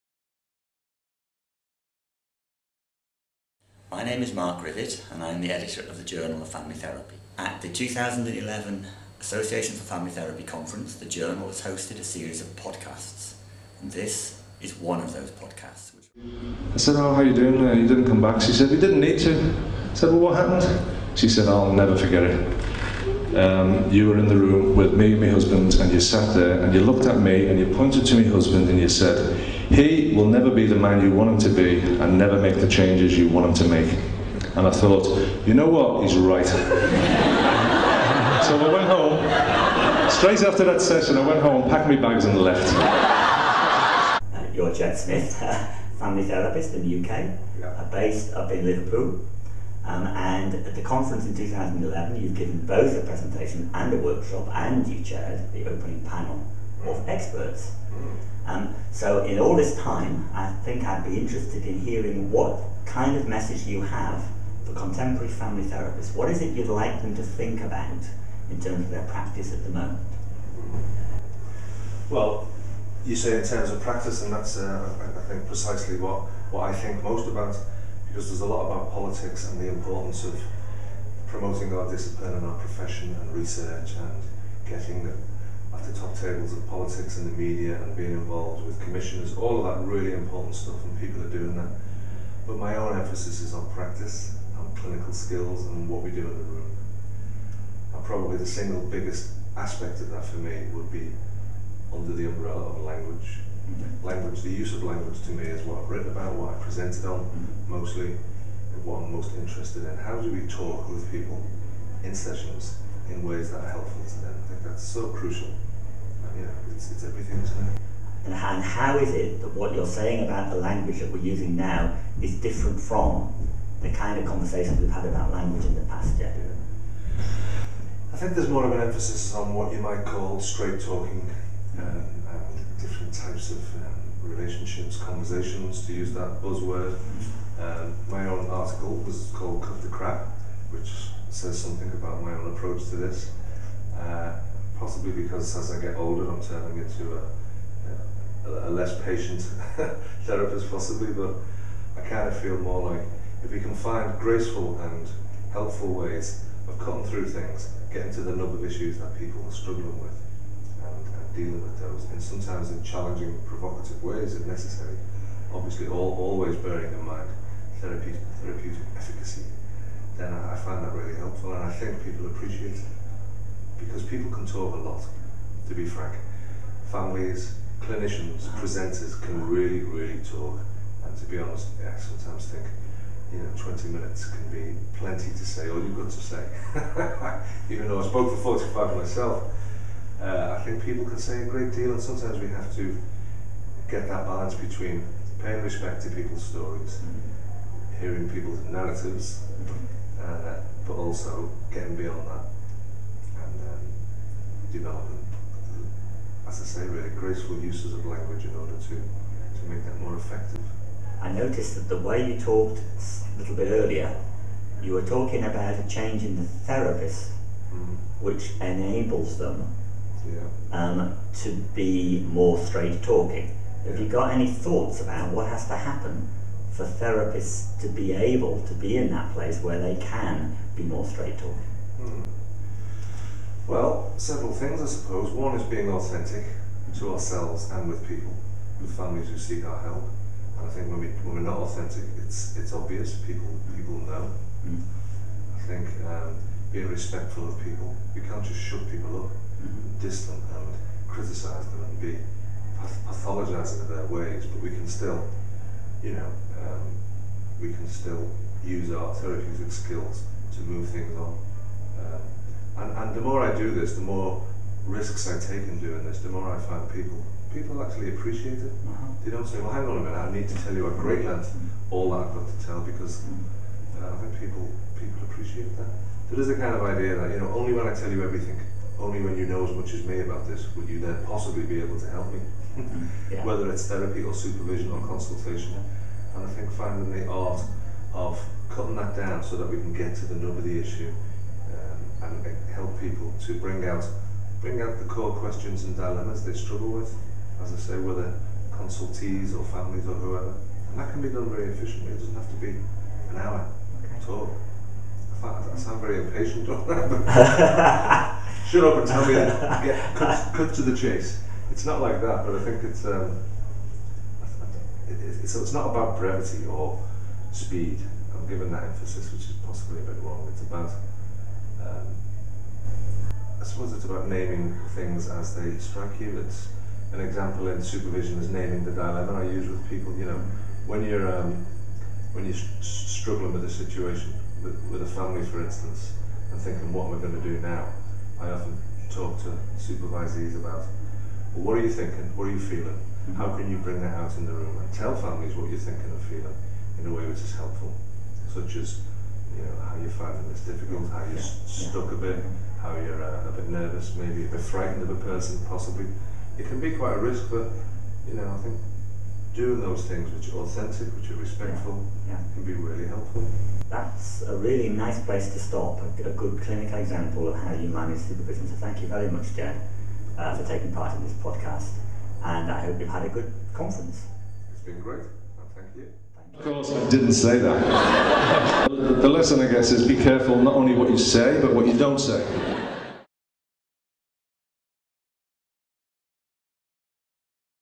Family Therapy & Systemic Practice: Ged Smith Interview by JFT Editor Mark Rivett
Ged Smith, family therapist at Liverpool, UK. He is also the deputy editor of Context, the magazine for family therapy and systemic practice in the UK. He was interviewed by Mark Rivett, Editor of Journal of Family Therapy, on 23/9/2011 at the AFT National Conference 2011, Buxton. Ged shared his views about "Cut the crap: Language - risks and relationships in systemic therapy and supervision" which he wrote in the 116 August 2011 issue of Context.

Journal of Family Therapy (JFT) is a journal published for the Association for Family Therapy and Systemic Practice in UK.